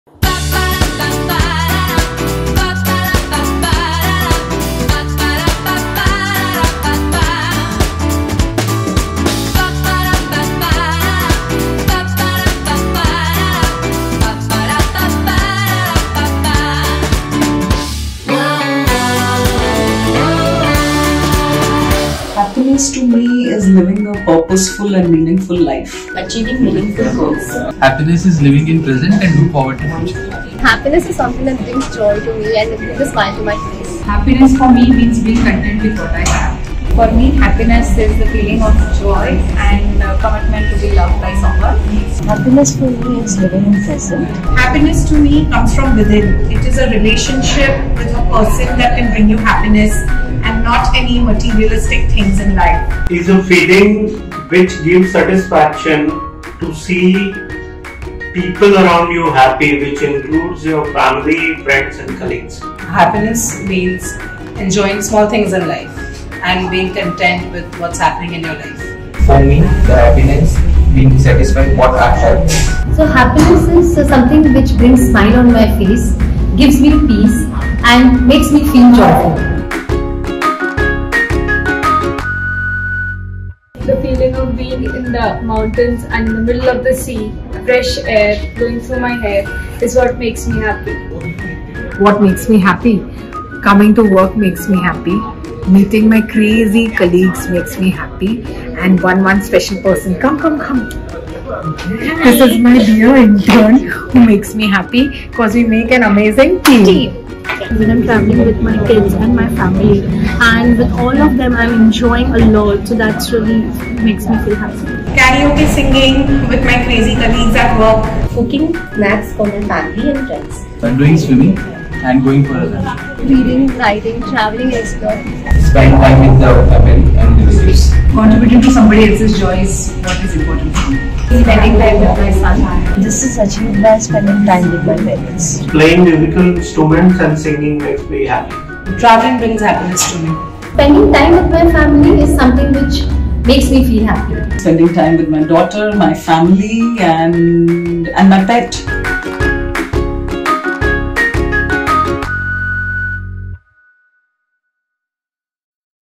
😊 Happy World Happiness Day 😊 | Future Generali India Life Insurance
Happiness is contagious and our team members at Future Generali India Life Insurance prove it! On the occasion of the International Day of Happiness, we asked them to share what happiness means to them and what makes them happy. Watch this fun video and catch some of their happy vibes! We wish you a day full of happiness and beyond.